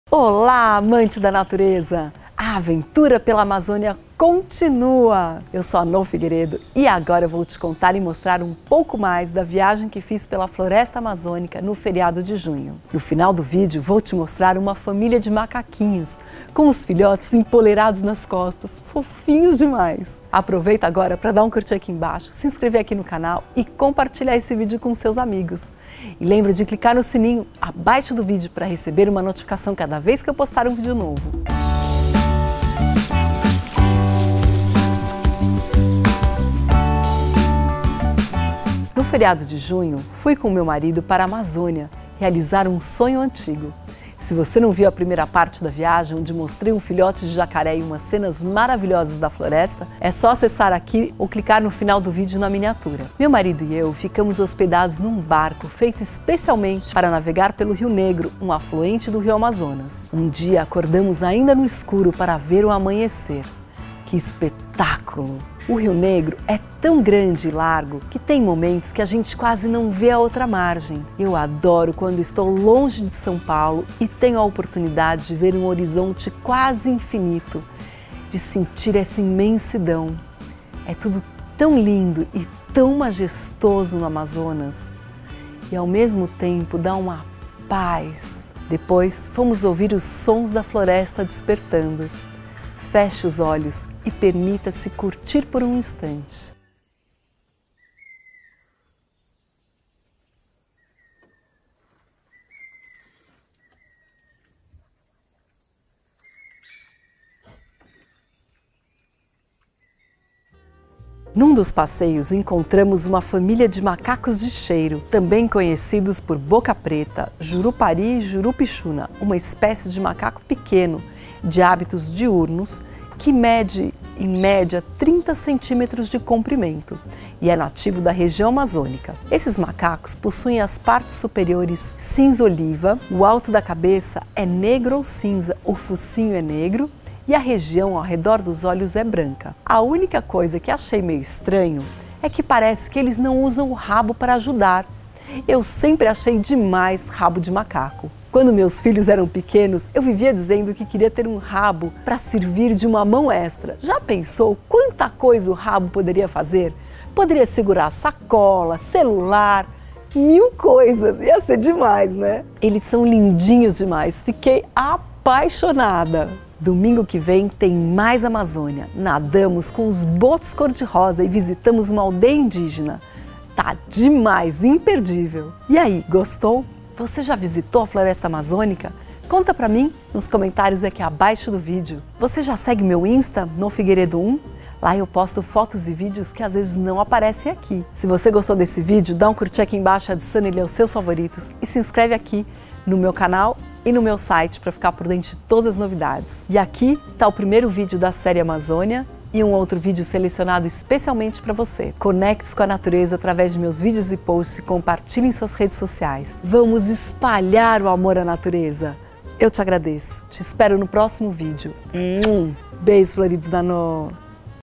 Vendo o Dia Nascer em Pleno Rio Negro | Especial Amazônia 02| Nô Figueiredo
Vendo o Dia Nascer em Pleno Rio Negro

Nesse vídeo a Nô Figueiredo conta mais sobre a viagem que fez com seu marido, Celso, para a Floresta Amazônica em junho 2019.

Beijos floridos da Nô
Nô Figueiredo
paisagista, florista e youtuber
Conexão com a natureza